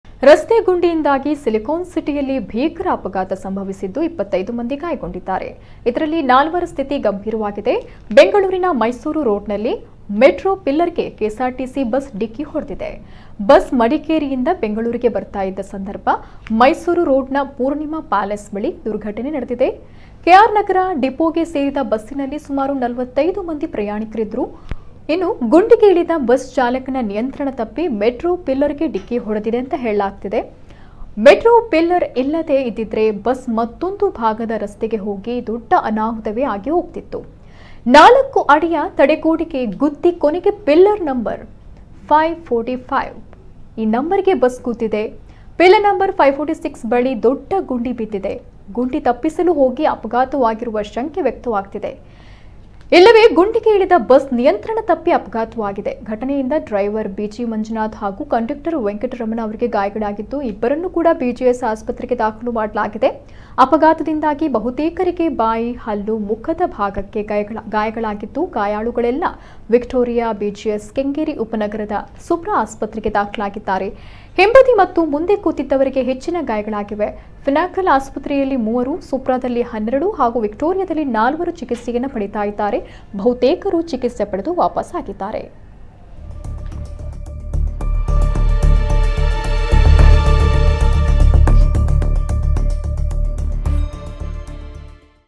ಬೆಂಗಳೂರಿನಲ್ಲಿ ಮೆಟ್ರೋ ಪಿಲ್ಲರ್‌ ಗೆ ಗುದ್ದಿದ ಸರ್ಕಾರಿ ಬಸ್..! 25 ಮಂದಿಗೆ ಗಾಯ
ರಸ್ತೆ ಗುಂಡಿಯಿಂದಾಗಿ ಸಿಲಿಕಾನ್ ಸಿಟಿಯಲ್ಲಿ ಭೀಕರ ಅಪಘಾತ ಸಂಭವಿಸಿದ್ದು, 25 ಮಂದಿ ಗಾಯಗೊಂಡಿದ್ದಾರೆ. ಇದರಲ್ಲಿ ನಾಲ್ವರ ಸ್ಥಿತಿ ಗಂಭೀರವಾಗಿದೆ.ಬೆಂಗಳೂರಿನ ಮೈಸೂರು ರೋಡ್ ನಲ್ಲಿ ಮೆಟ್ರೋ ಪಿಲ್ಲರ್‍ಗೆ ಕೆಎಸ್‍ಆರ್ ಟಿಸಿ ಬಸ್ ಡಿಕ್ಕಿ ಹೊಡೆದಿದೆ. ಬಸ್ ಮಡಿಕೇರಿಯಿಂದ ಬೆಂಗಳೂರಿಗೆ ಬರುತ್ತಿದ್ದ ಸಂದರ್ಭದಲ್ಲಿ ಮೈಸೂರು ರೋಡ್‍ನ ಪೂರ್ಣಿಮಾ ಪ್ಯಾಲೇಸ್ ಬಳಿ ದುರ್ಘಟನೆ ನಡೆದಿದೆ